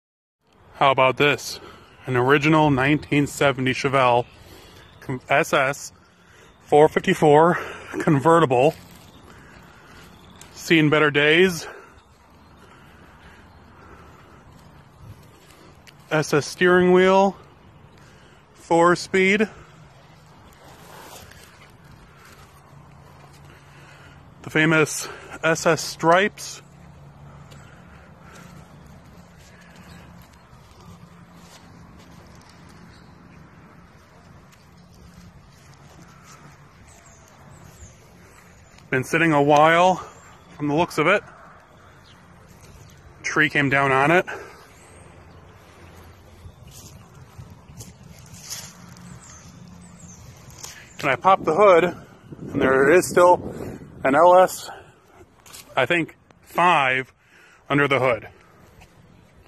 Chevelle ss barn find, would you keep it?
In this video, we're looking at a chevelle ss barn find – would you keep it? As a car collector, it's important to know what your car is worth and if it's a barn find – an extremely rare find – or not.

In this short video, we'll help you decide if you should keep or sell this amazing chevelle ss barn find. Watch the video and find out what you think!